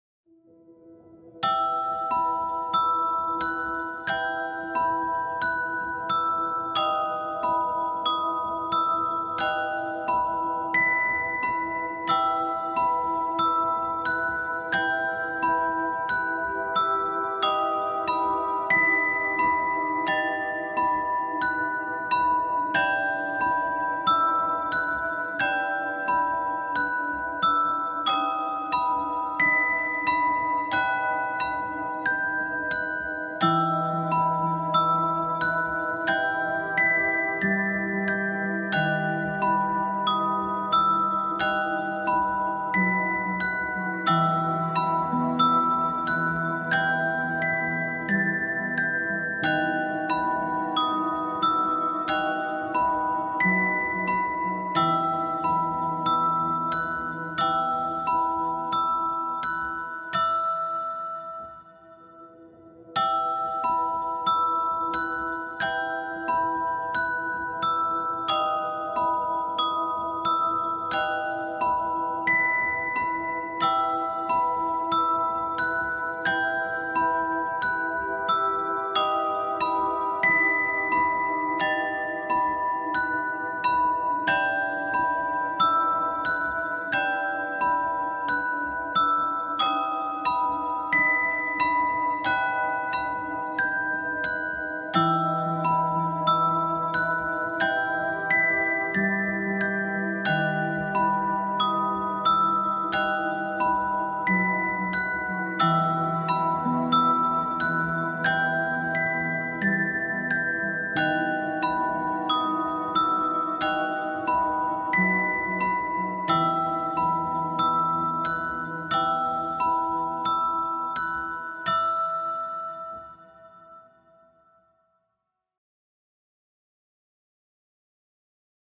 House Entertainment Center, scary and funny-I'm scared to shit
Развлекательный центр Wizard House, страшно и смешно Таиланд  город Паттайя